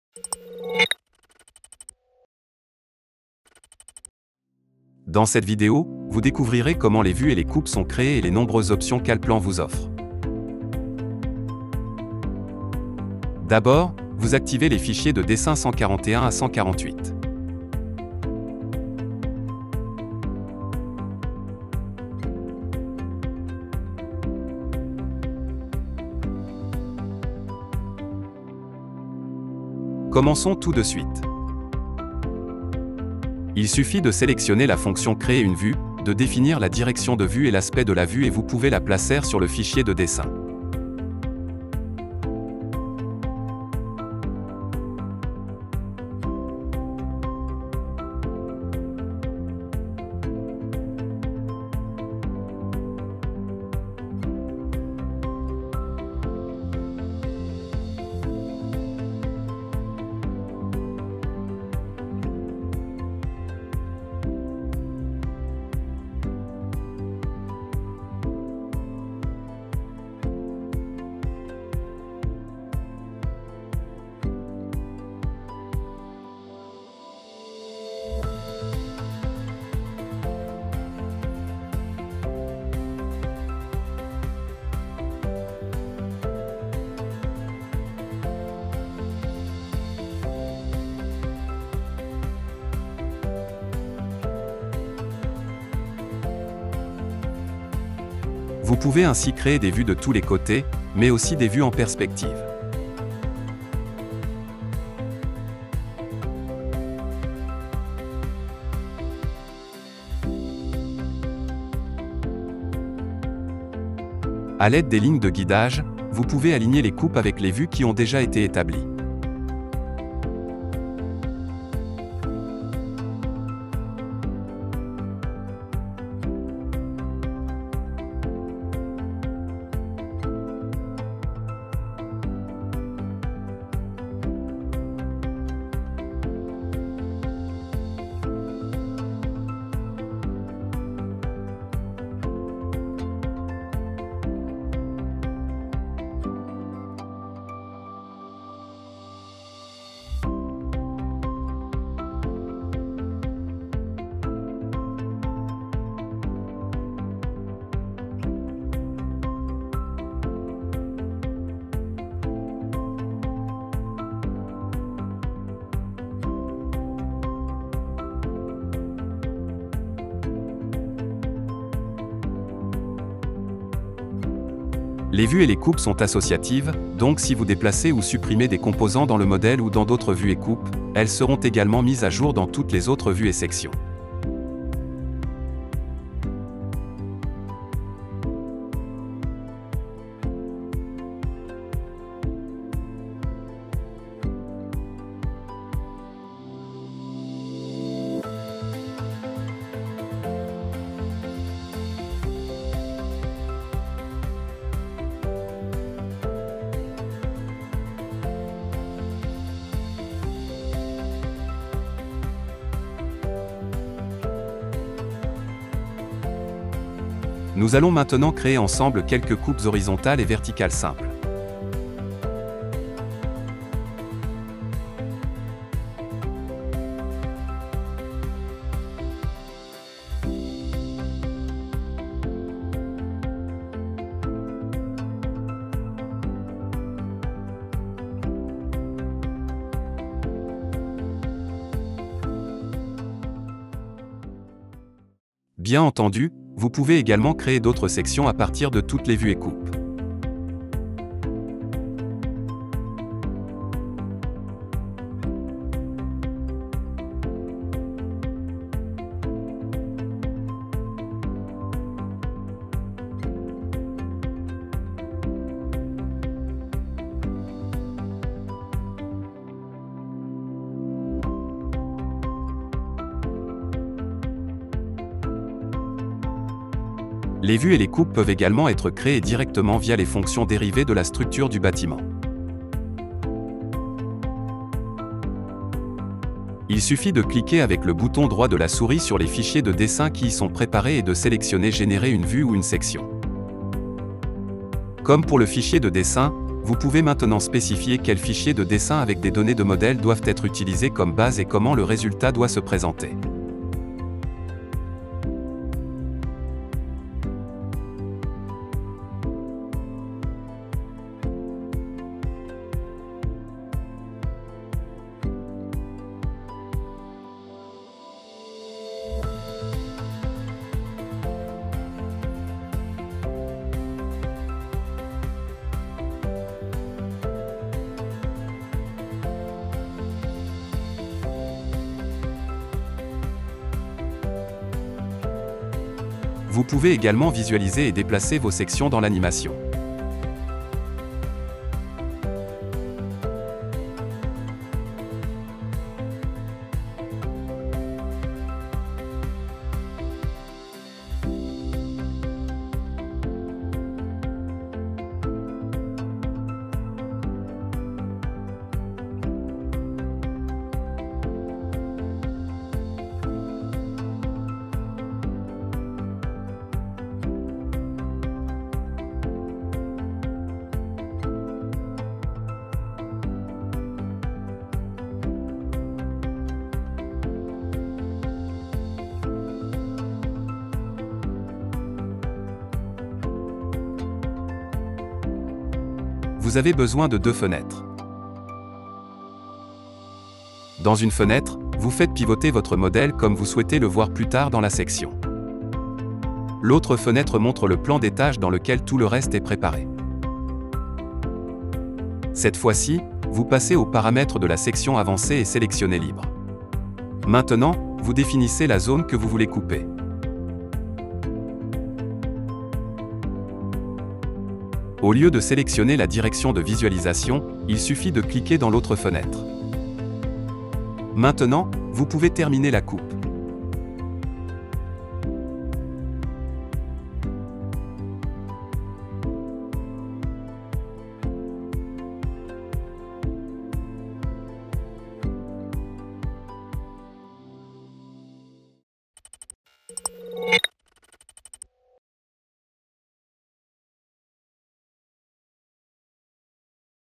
Hello Allplan! 2022 - VUES ET SECTIONS
Dans cette vidéo, vous découvrirez comment les vues et les coupes sont créées et les nombreuses options qu'Allplan vous offre.

0:00:00 - COMMENCER
0:00:12 - VUES
0:03:36 - COUPES
0:04:36 - VUES ET COUPES
0:05:35 - ANIMATION
0:06:25 - COUPES ISOMÉTRIQUES